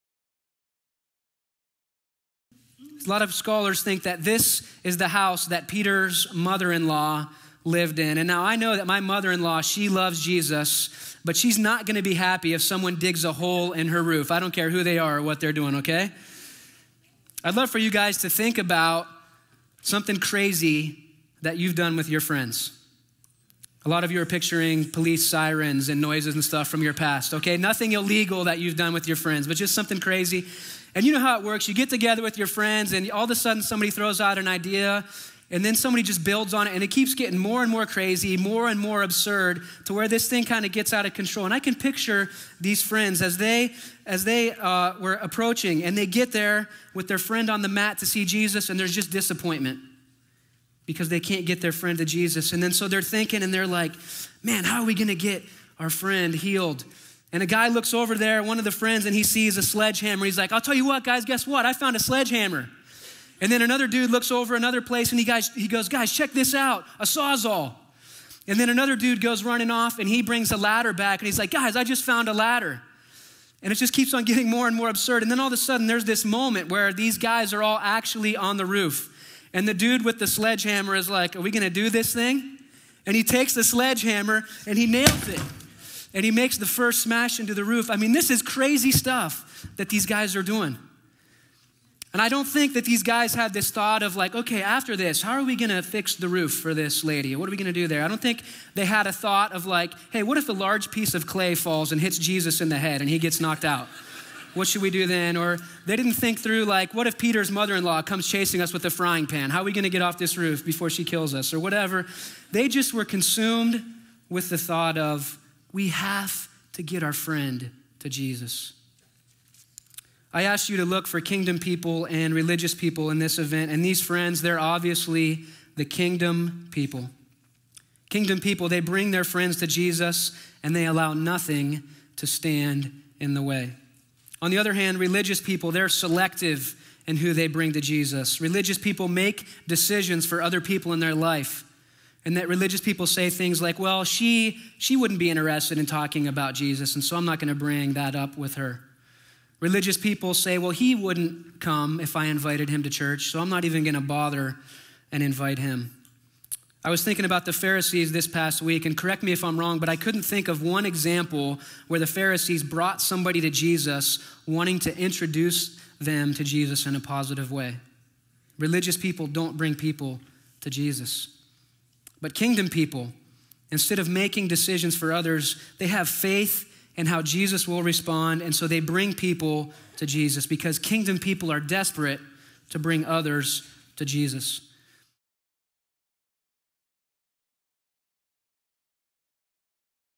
Matthew's Book / A Crazy Idea
In this clip, High School Pastor Brad Mock talks about the crazy idea that a group of friends followed to get their paralyzed friend healed by Jesus.

For more information about Christ Community Church, visit the You can also watch the service